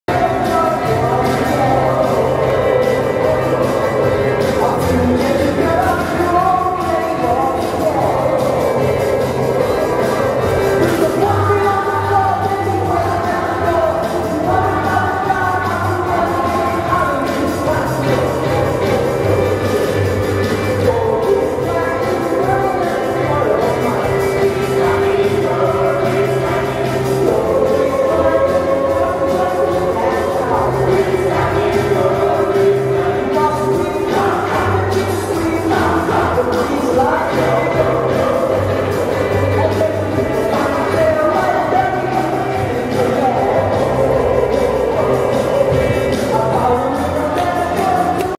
Berkeley's High School Musical May 2017 3
Berkeley's high school put on an amazing show this month. congratulations to all the cast on a wonderful set of performances.